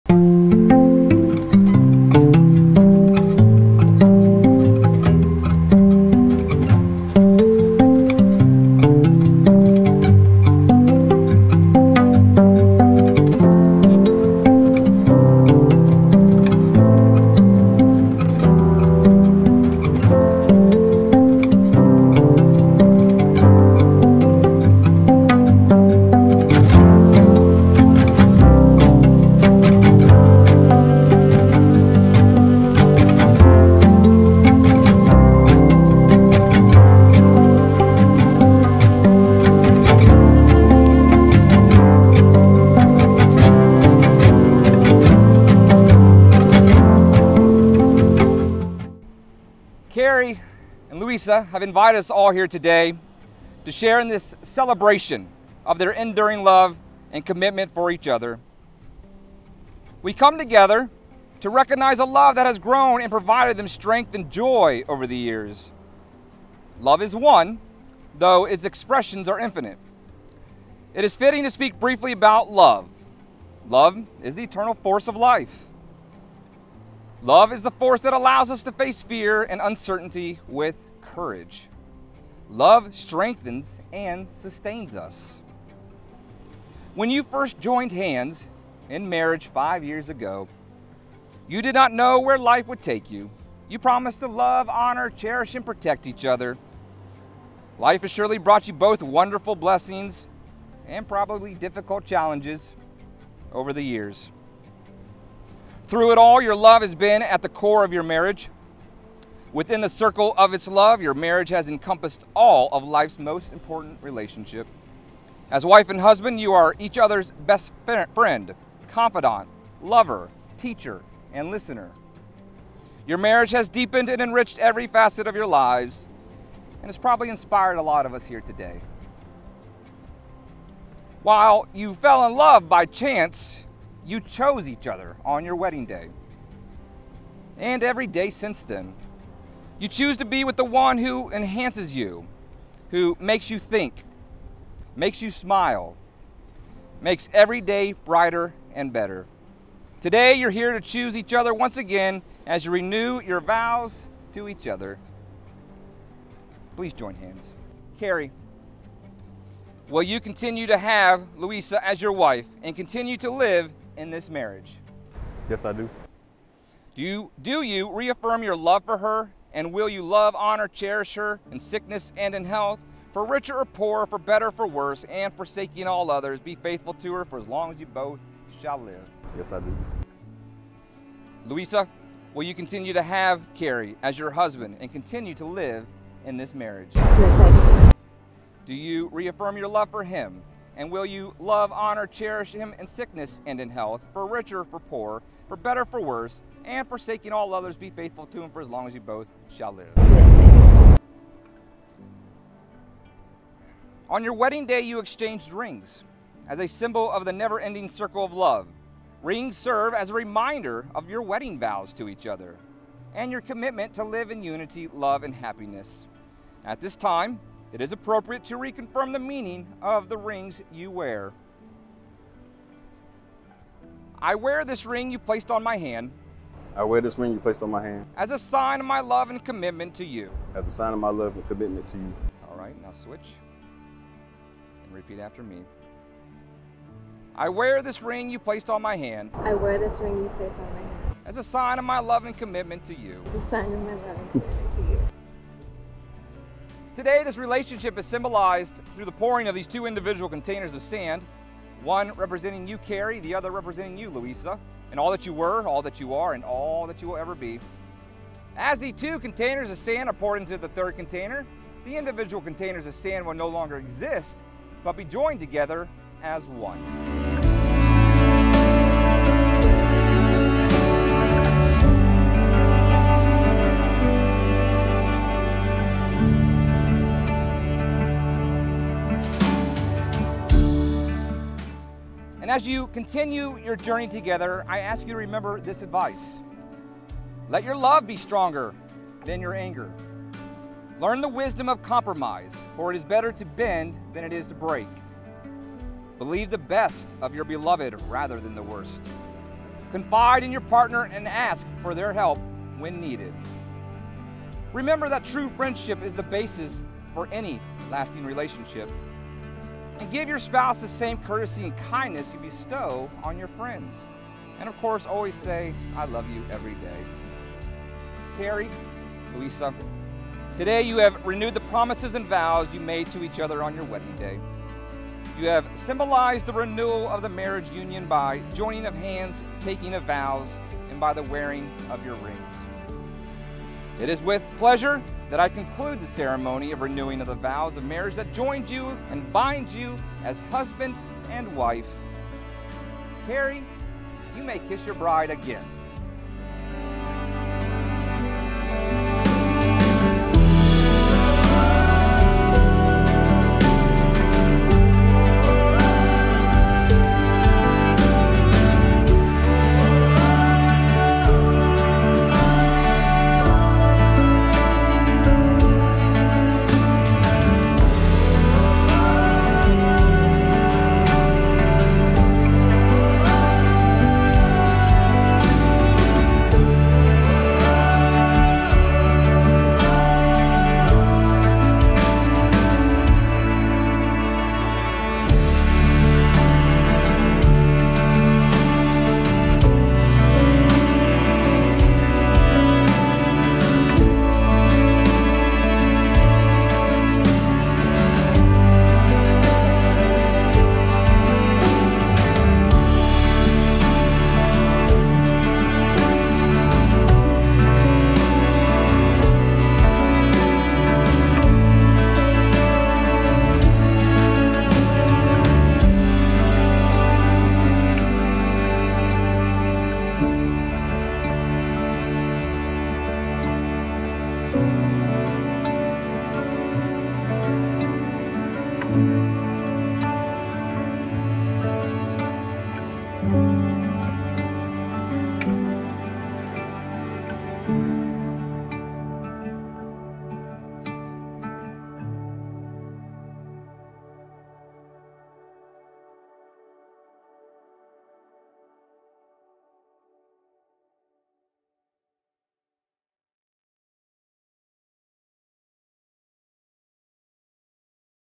Kerry and Luisa - September 23, 2024
Kerry and Luisa's vow renewal at the beach in North Myrtle Beach, SC.

Barefoot Wedded Bliss